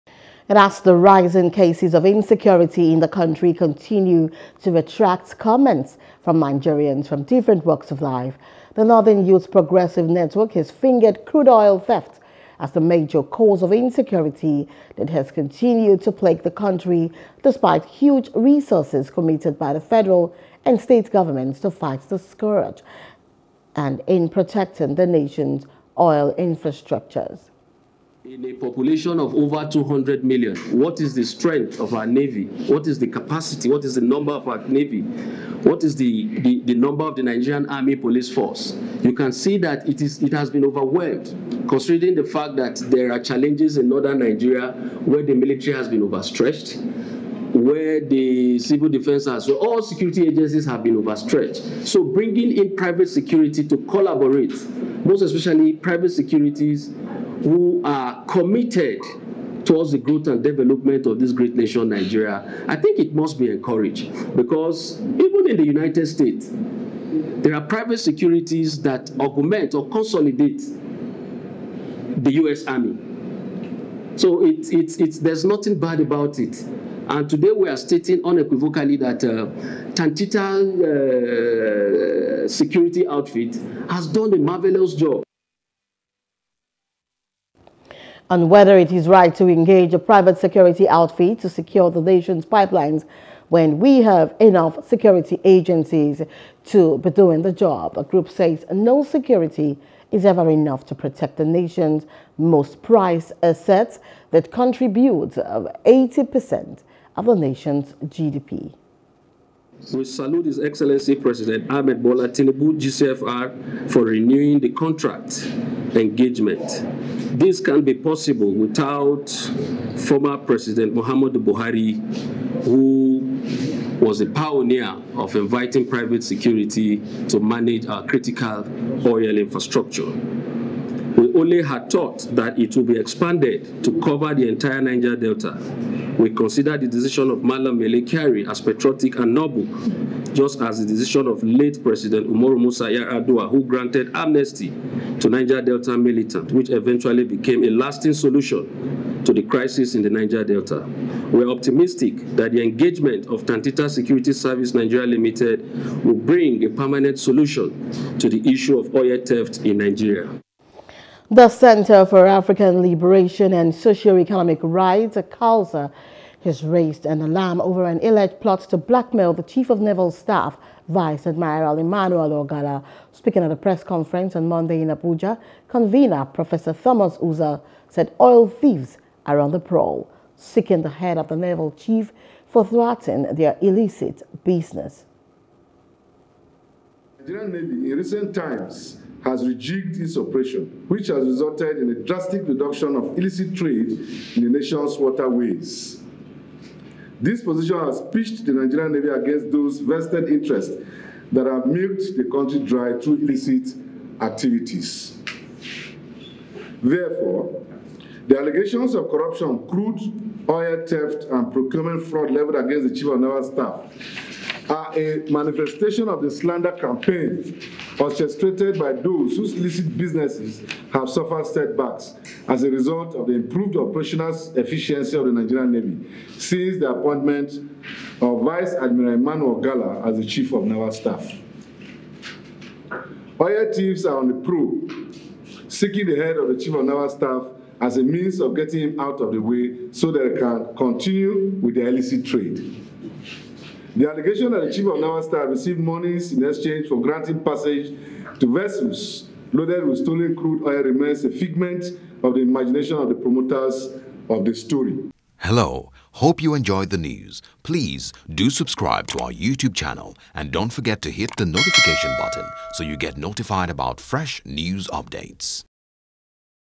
Oil Theft: Northern Group Links Case To Rising Insecurity
As the rising cases of insecurity in the country continue to attract comments from Nigerians from different works of life ,the Northern Youths Progressive Network has fingered crude oil theft as the major cause of insecurity that has continued to plague the country despite huge resources committed by the federal and state governments to fight the scourge and in protecting the nations oil infrastructures.